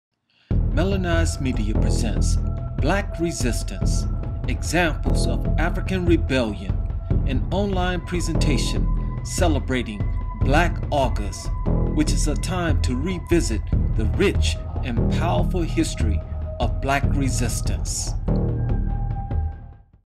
Black Resistance: Examples of African Rebellion
"Black Resistance: Examples of African Rebellion," an online presentation celebrating Black August which is a time to revisit the rich history of Black resistance.